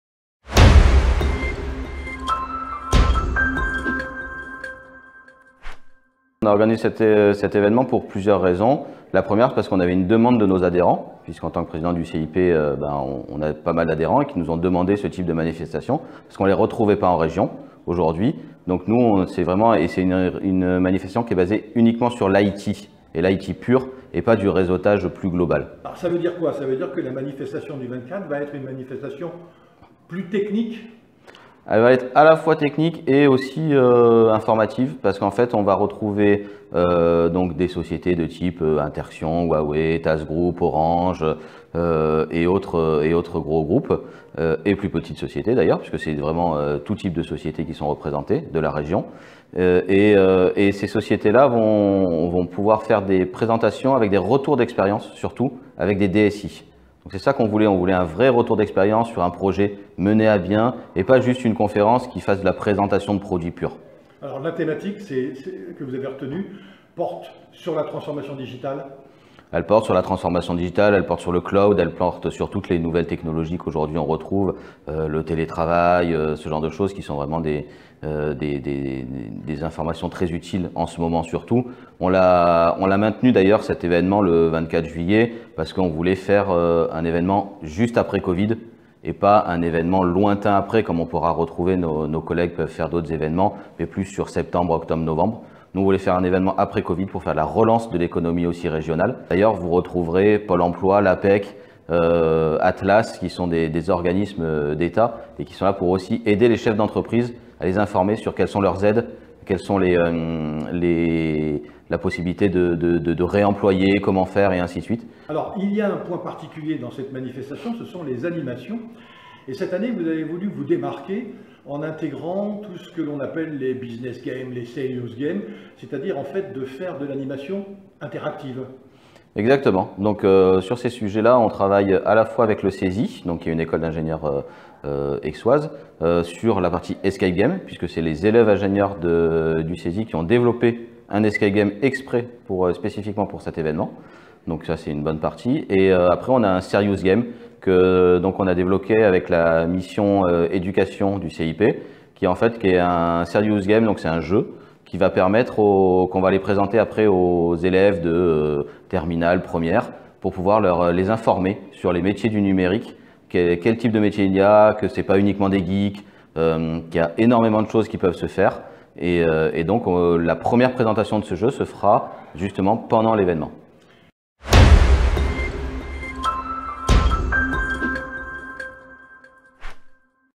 Guillaume Goury Président du CIPMed
Guillaume Goury Président du CIPMed sur l'événement CIP Network Show du 24 juillet 2020 à l'Orange Vélodrome de Marseille.